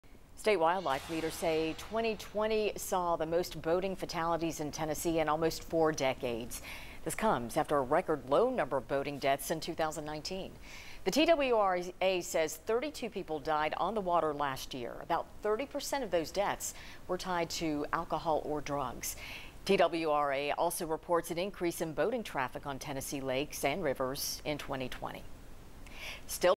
TWRA: 2020 saw most boating deaths in 37 years
State wildlife leaders say 2020 saw the most boating fatalities in Tennessee in almost four decades.